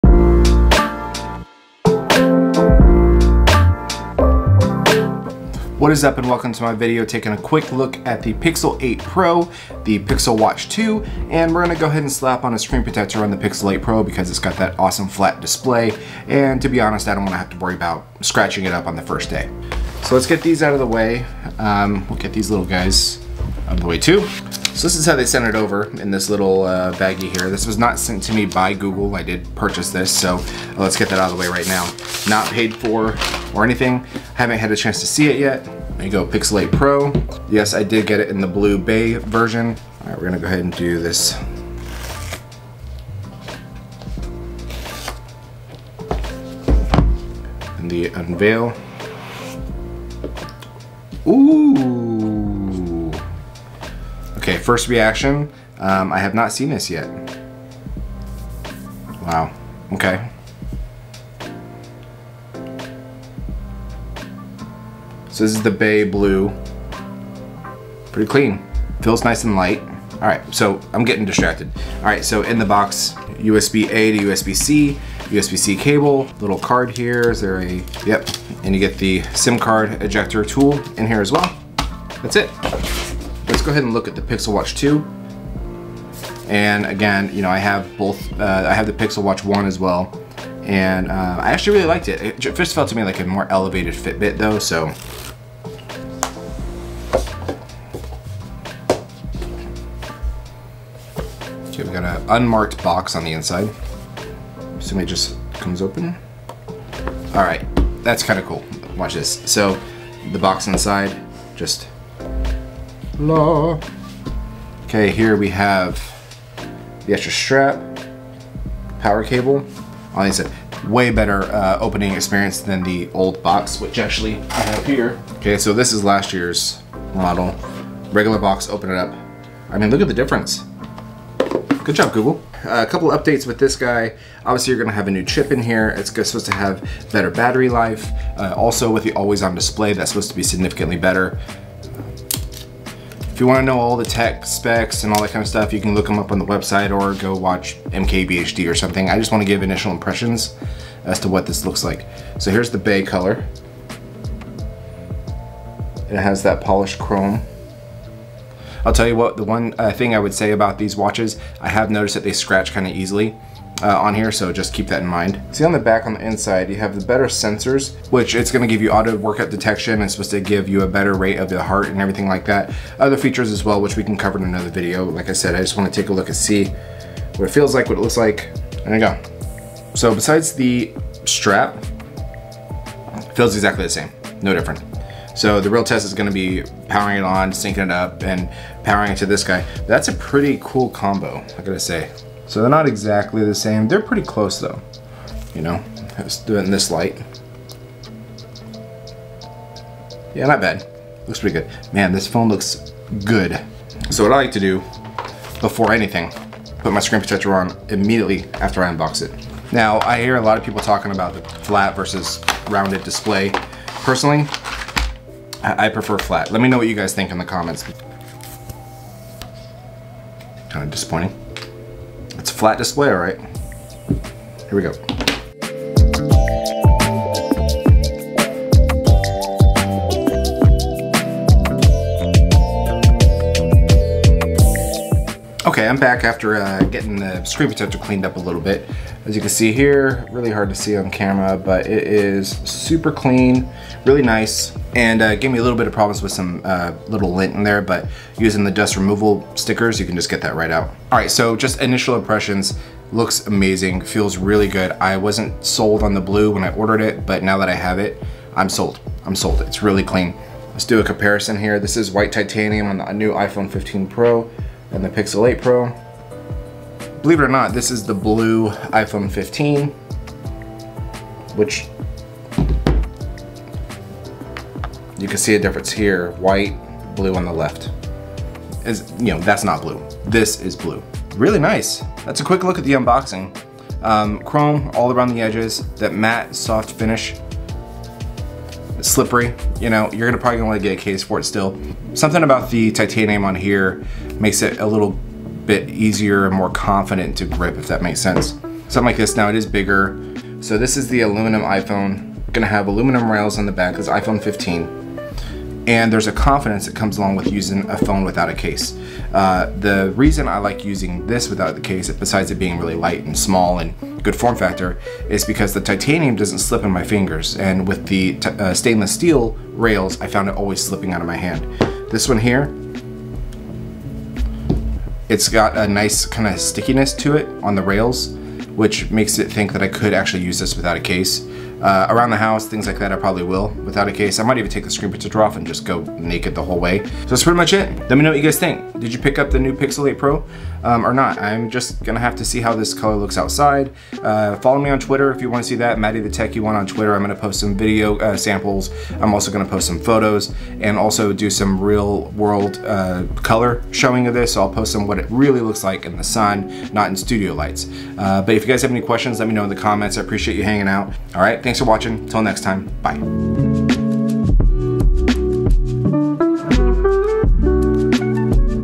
PIXEL 8 PRO & PIXEL WATCH 2 BAY BLUE
My first impressions on the new BAY Blue Pixel 8 Pro and Pixel Watch 2, and the first look at the Spine Screen Protector (not an ad)